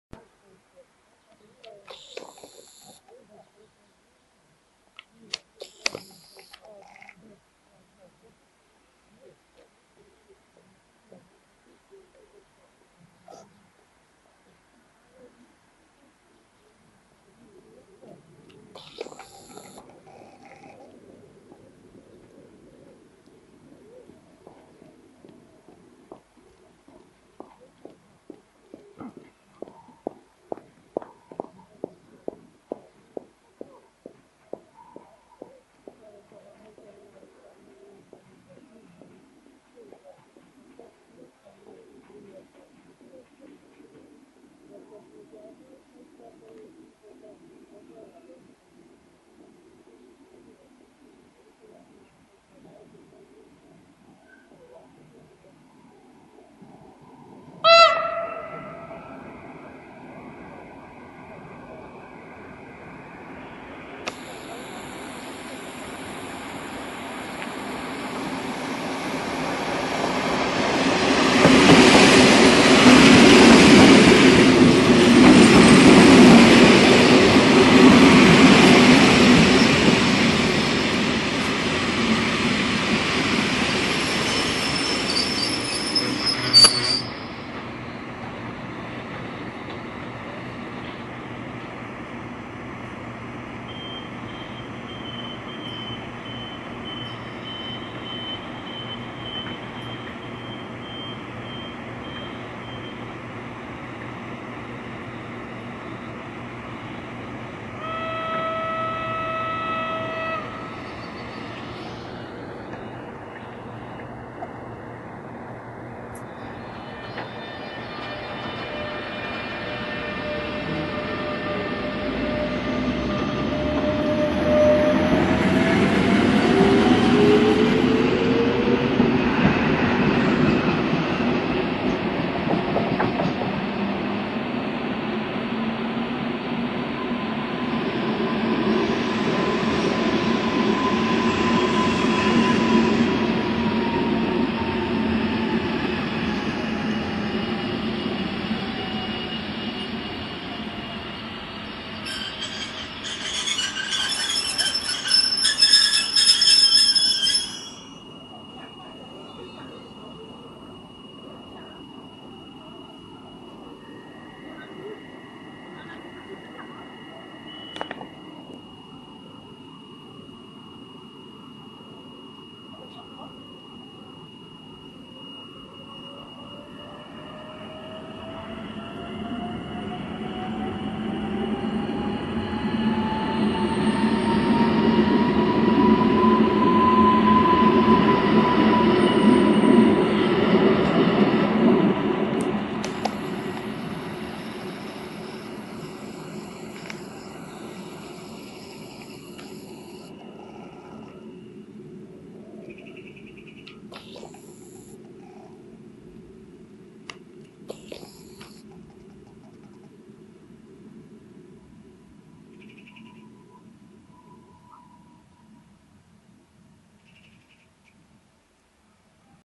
Mijanka starszych i nowszych EN57AKM - 2x RP1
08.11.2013. Poranna mijanka na p.o Świder:
EN57AKM-1564+EN57AKM-1573 jako pociąg osobowy KM 701 z Dęblina do Warszawy Zachodniej (RP1).
EN57AKM-1601+EN57AKM-1604 jako pociąg osobowy KM 312 z Łowicza Głównego do Otwocka.